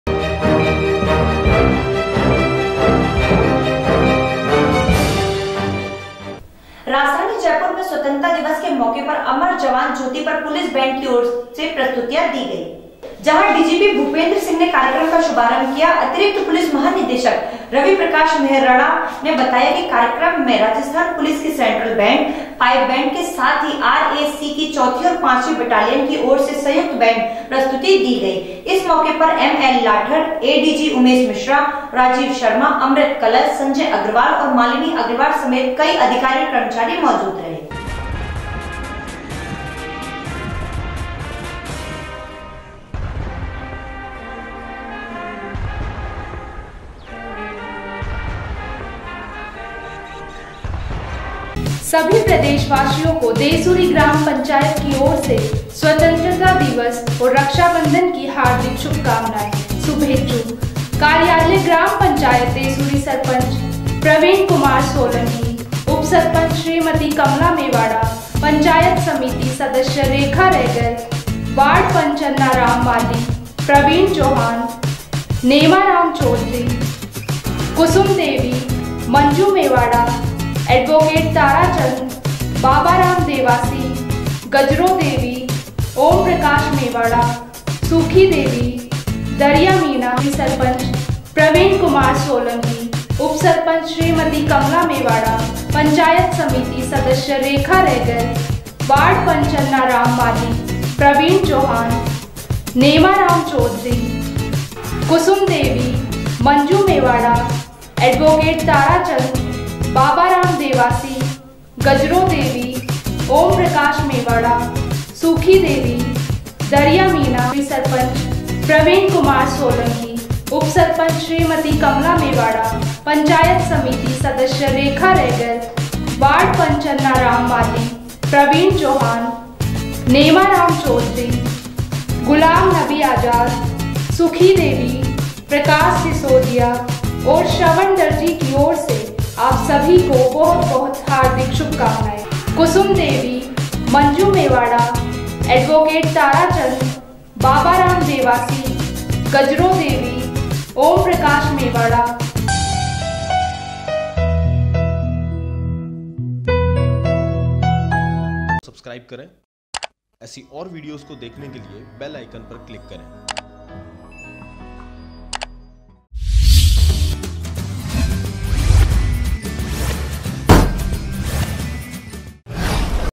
जयपुर अमर जवान ज्योंति पर पुलिस बैंड की प्रस्तुति
डीजीपी भूपेंद्र सिंह ने कार्यक्रम का शुभारंभ किया...अतिरिक्त पुलिस महानिदेशक रवि प्रकाश मेहरडा ने बताया कि कार्यक्रम में राजस्थान पुलिस के सेंट्रल बैंड. पाइप बैंड...के साथ ही आर ए सी की चौथी और पांचवी बटालियन की ओर से संयुक्त बैंड प्रस्तुति दी गई. इस मौके पर एम एल लाठर. एडीजी उमेश मिश्रा. राजीव शर्मा. अमृत कलश. संजय अग्रवाल और मालिनी अग्रवाल समैत कई अधिकारी और कर्मचारी मौजूद रहे।